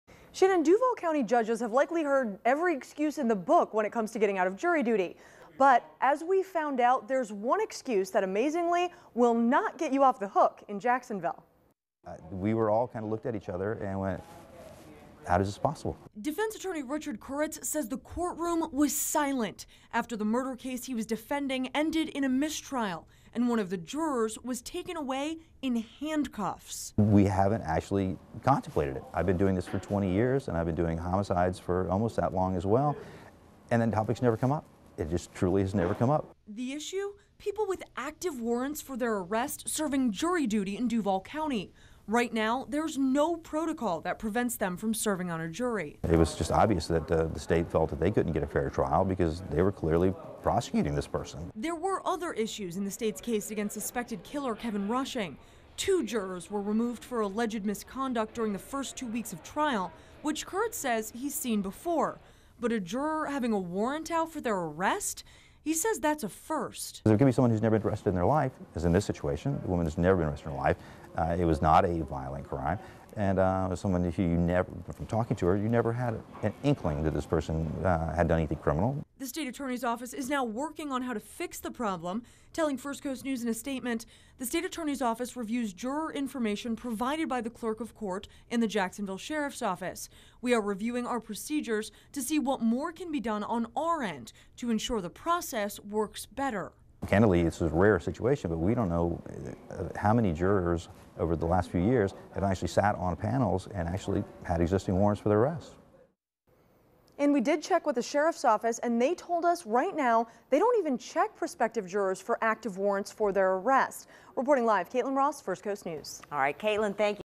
One excuse that won't get you out of jury duty in Duval County? A warrant for your arrest.
A loophole in the Duval County Court system allows people with active warrants for their arrest to serve jury duty.